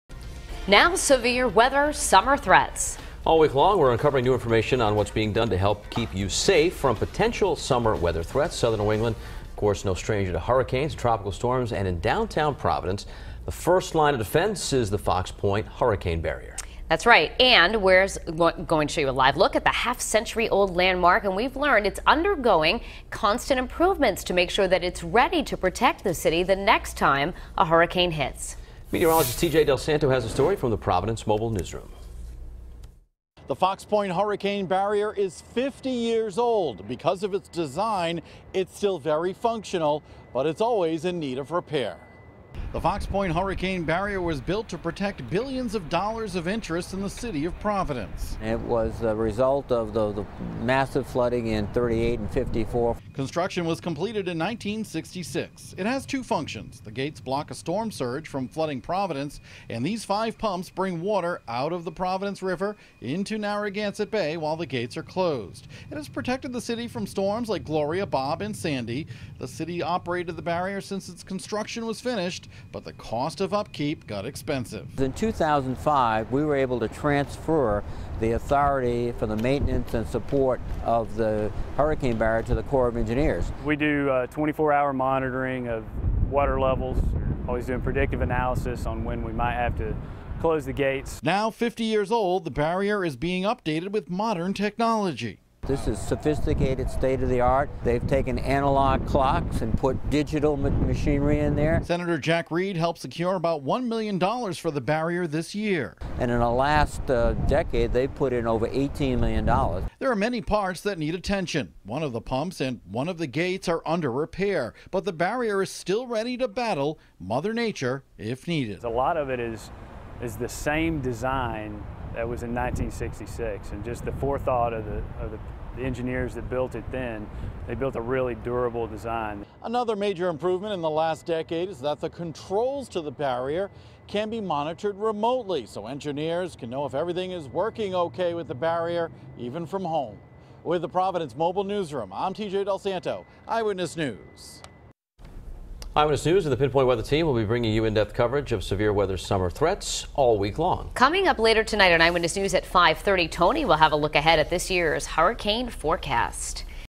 Severe Weather Week: Fox Point Hurricane Barrier
The half-century old landmark is undergoing constant improvements so it's ready to protect the city next time a hurricane hits.